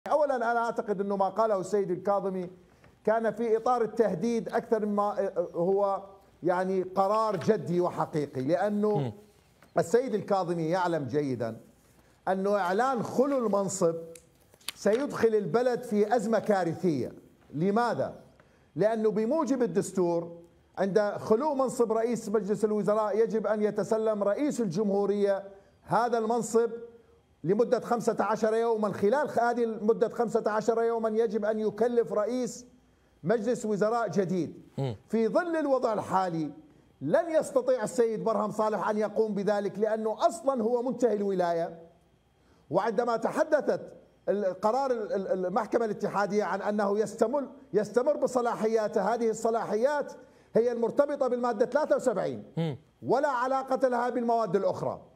الباحث السياسي يحيى الكبيسي: إعلان الكاظمي بالاستقالة كان في إطار التهديد
الباحث السياسي يحيى الكبيسي: إعلان الكاظمي بالاستقالة كان في إطار التهديد لعلمه بأن خلو منصب رئيس الوزراء سيُدخل البلد في كارثة جديدة

شارك برأيك عبر التعليقات أسفله وساهم في إثراء النقاش

حمل تطبيق "الحرة كونيكت" للتلفزيونات والهواتف الذكية:
أمازون فاير